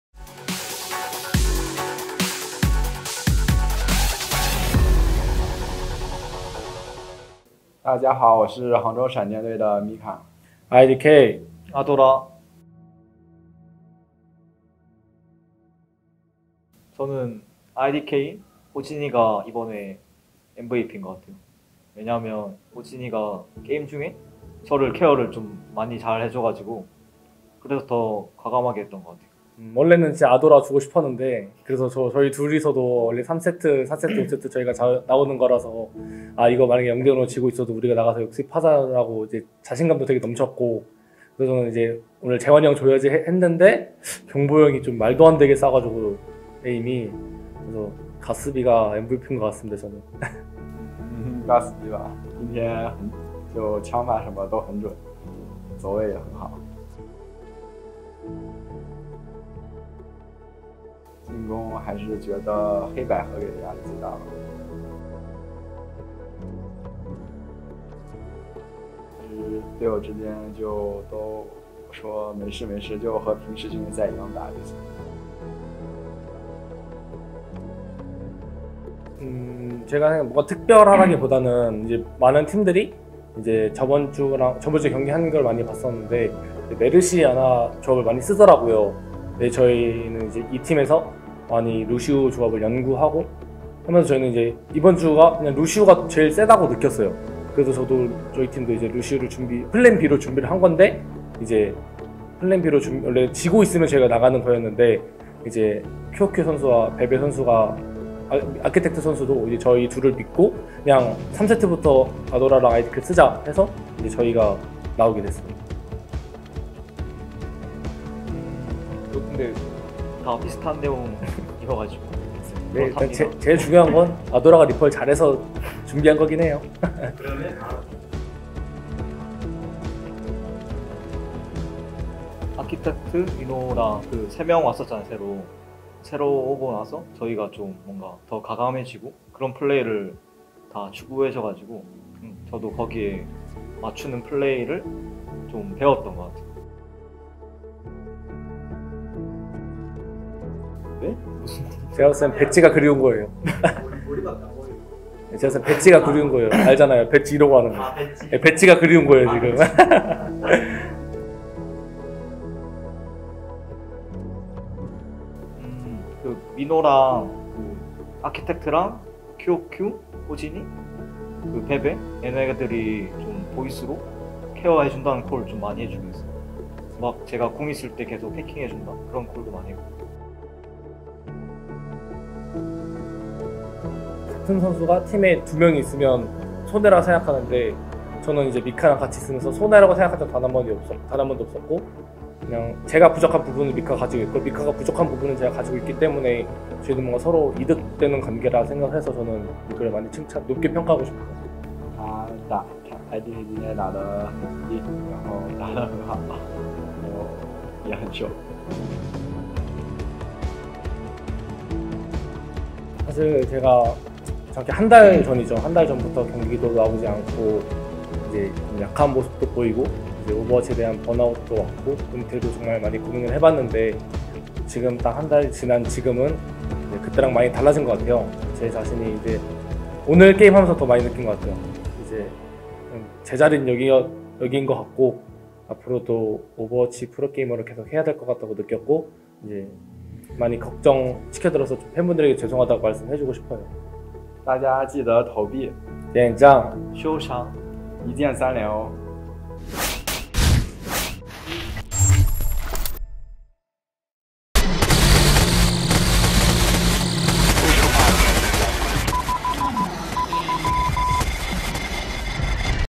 Spark Talks 0825: The Story About The Lucio & Reaper Comp
M1KA, IDK & ADORA reviewed the match and shared their stories from behind the scenes. Watch the video and find out what happened with the Lucio & Reaper comp. (^u^)

A series of puzzles are placed related to some HUGE news starting with this video. Join us and find out!